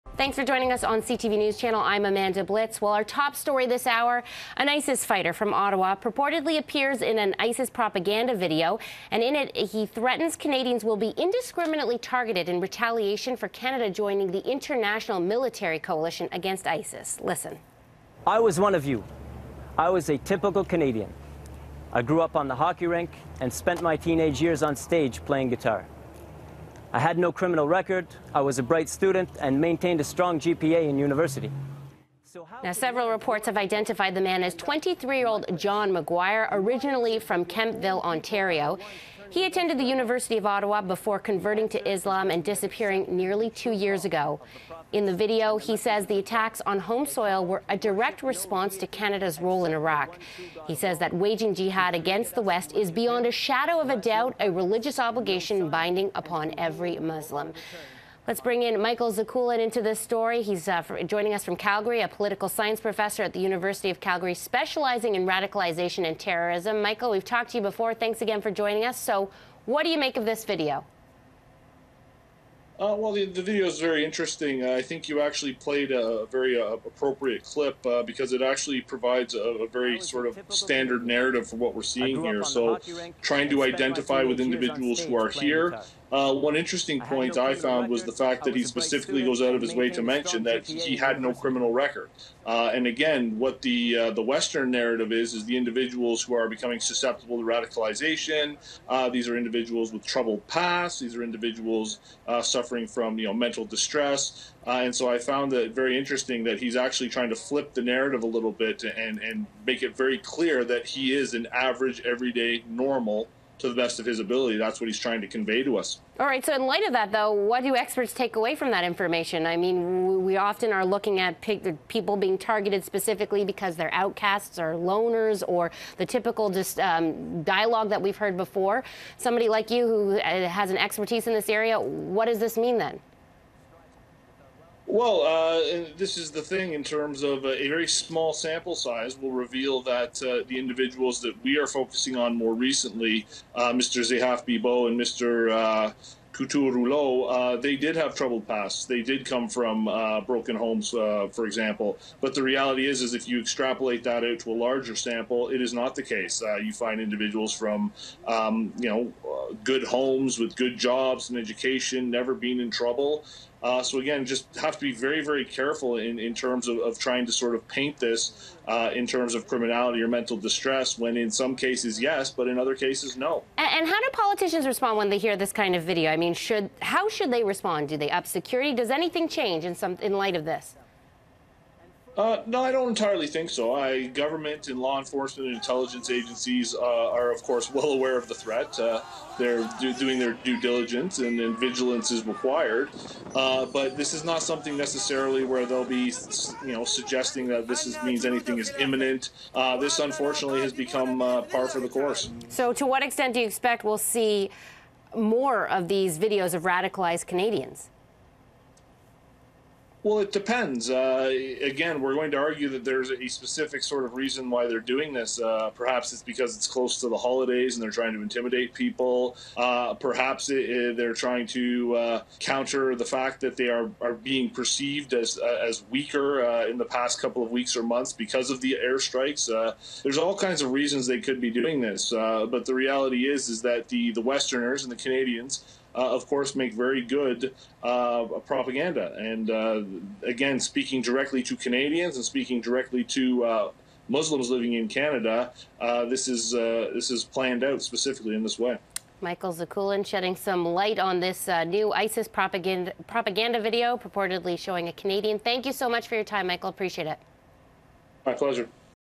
Canadian man in purported ISIS video threatens revenge against Canada CTV News
CTVNews.ca Staff
Published Sunday, December 7, 2014 3:20PM EST
Last Updated Sunday, December 7, 2014 6:10PM EST

A purported new Islamic State propaganda video features a man who says he is Canadian and threatens violence and “punishment” against Canadians on their own soil.

In the video, distributed by a U.S. company that monitors jihadist posts around the world, the man who identifies himself as Abu Anwar al-Canadi says others should follow the example of Martin Couture-Rouleau, who ran down and killed Warrant Officer Patrice Vincent in Quebec nearly two months ago.

The man also references the attack on Parliament Hill and the National War Memorial, in which Cpl. Nathan Cirillo was killed.

SITE Intelligence Group, a U.S.-based company that monitors trends within the global jihadist movement, says Islamic State has released a video calling for lone-wolf attacks on Canadian targets. On the video is a man who says he is a Canadian and identifies himself as 'Abu Anwar al-Canadi.' (Handout / THE CANADIAN PRESS)

Standing in the ruins of an unidentified town, the man rails against Canada’s involvement in the U.S.-led airstrikes against ISIS targets in Iraq.

“It should not surprise you when operations by the Muslims are executed where it hurts you the most – on your very own soil,” he says in the video, which is just over six minutes long.
“Your people will be indiscriminately targeted, as you indiscriminately target our people.”

Looking into the camera, the man says: “I was one of you. I was a typical Canadian.”

He says he “grew up on the hockey rink,” had a high GPA in university, and had no criminal record until he converted to Islam and accepted “the true call of the prophets and the messengers of God.”

Addressing Muslims living in Canada, the man says they have two options: either pack up and join the Islamic State, or follow Couture-Rouleau’s example.

CTV News has not been able to independently verify the video. It’s not clear when it was recorded.

In response to the video, Public Safety Minister Steven Blaney issued a statement Sunday reaffirming Canada’s commitment to the anti-ISIS coalition.

“Terrorism remains a real and serious threat to Canadians, which is why we must remain vigilant,” Blaney said.

“That is why we are taking part in the coalition that is currently conducting air strikes against ISIL, and supporting the security forces in Iraq in their fight against this terrorist scourge.”

Previously posted ISIS propaganda videos have also called for violence against Canadians and other American allies.

Terrorism expert Michael Zekulin, a political science professor at the University of Calgary, says the latest video seems to be trying to “flip the narrative” of a troubled, young Canadian joining ISIS.

By explicitly stating that he was a good student with no criminal record, the man in the video is trying to make a point about what kind of Canadian could turn against his own country, Zekulin told CTV News Channel.